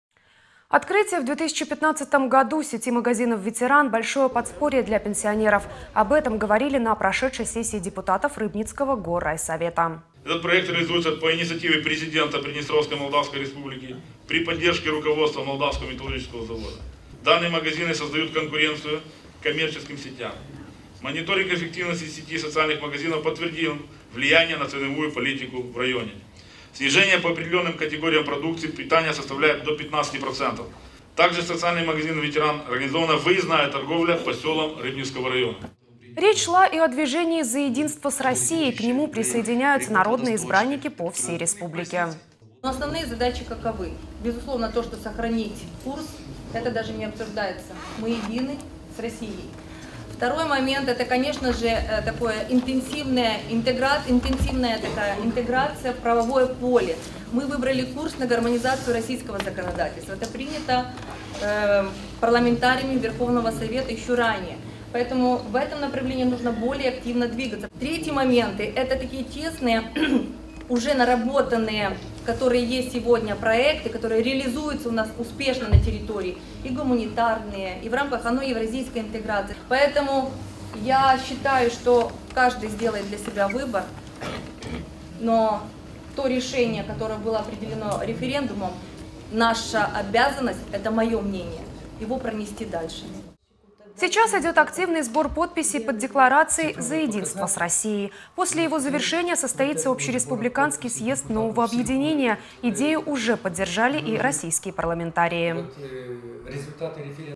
Движение «За единство с Россией» обсудили в Рыбнице
Открытие в 2015-м году сети магазинов «Ветеран» – большое подспорье для пенсионеров. Об этом говорили на прошедшей сессии депутатов Рыбницкого горрайсовета.